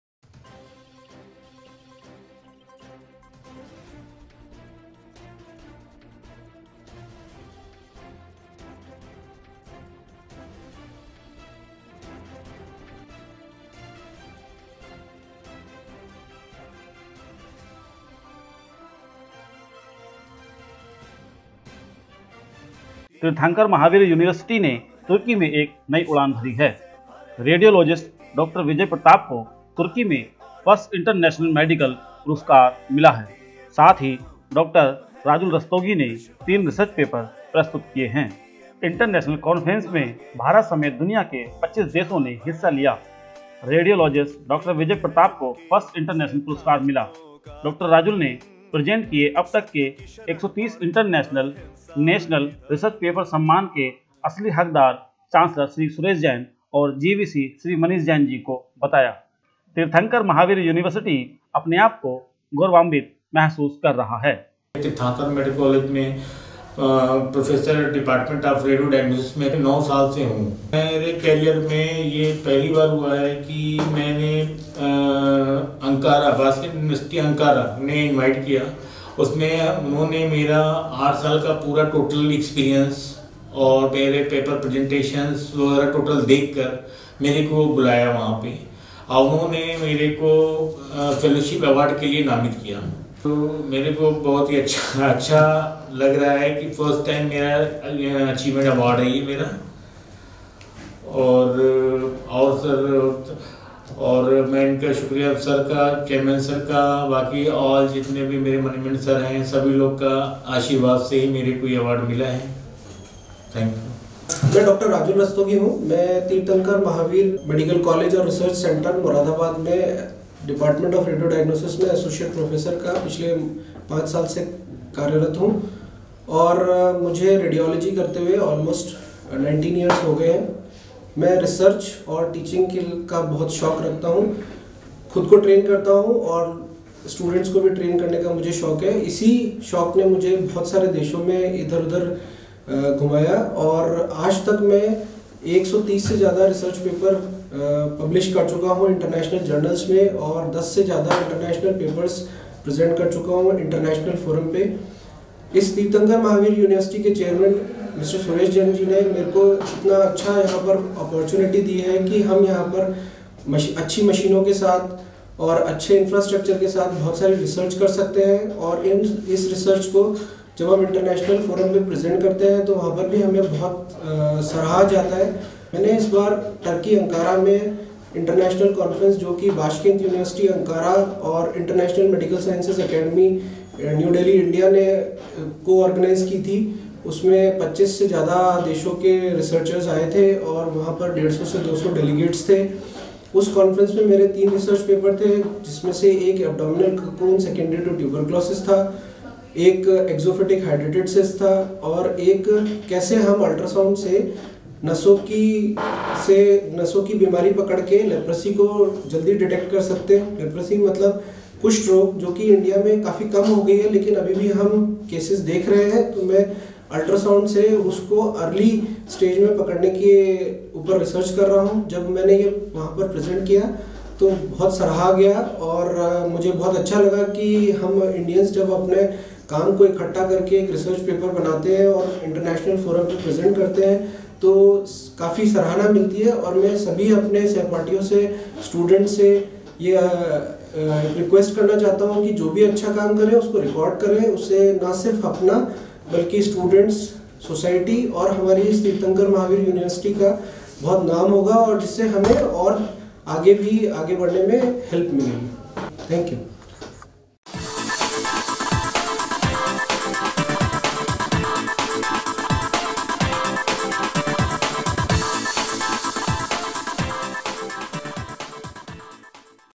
One more international award added in Teerthanker Mahaveer Medical College and Research Center
TMU :: One more international award added in Teerthanker Mahaveer Medical College and Research Center